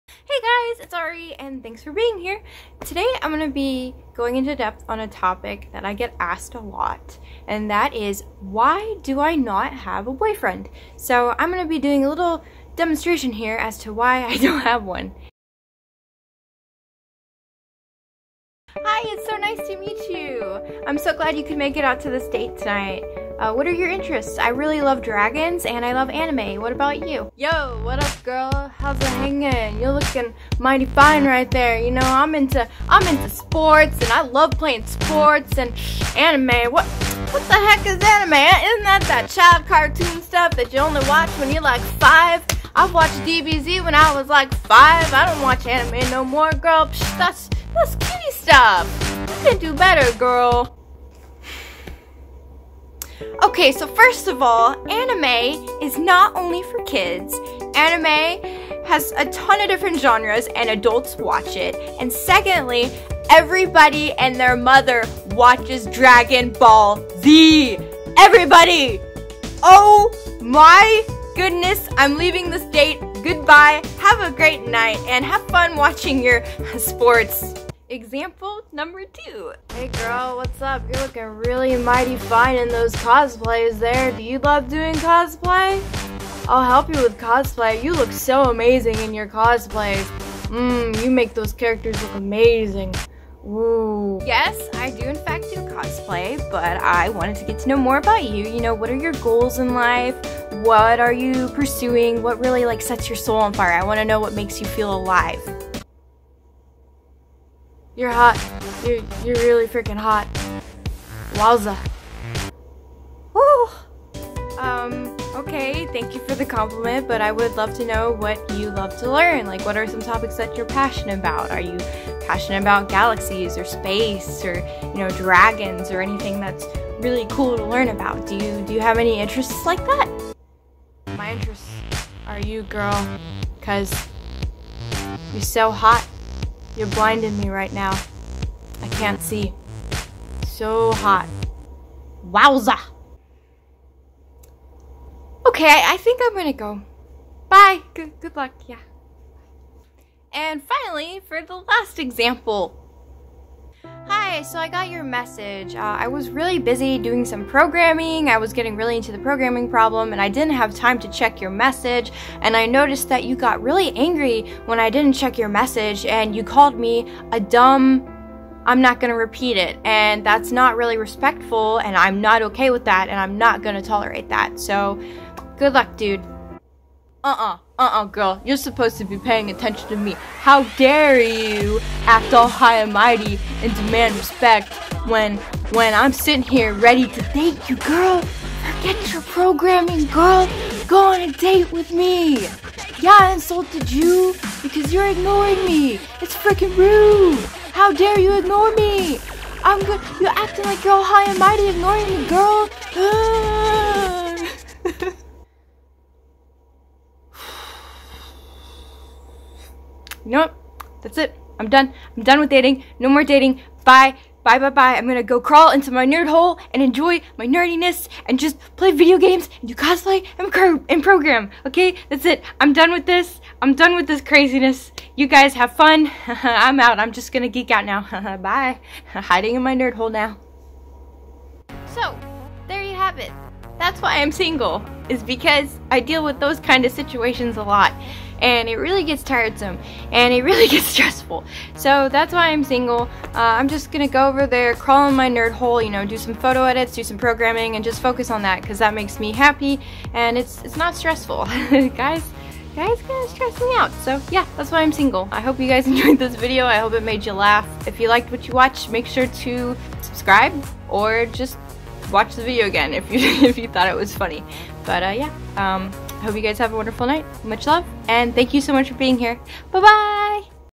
Why am I single?
Hey! It's Ari and thanks for being here :)

It's been a while since I've posted, but as for content I'm thinking of creating videos on irl topics and web development when I eventually get good at web development.

In this video I'm covering a question I get asked all the time. I thought I'd go ahead and make this and just send this video to whoever asks why I'm single in the future lol!

These examples in the video are based on real life events and have actually happened before, and are why I give up on dating so often.

But at the end of the day the biggest reason as to why I give up so much is because I love all my hobbies so so much, and it's so difficult searching for another human being that makes me as happy as my hobbies and this is a big reason why I give up all the time.

I hope you enjoy this video :) Thank you for being here!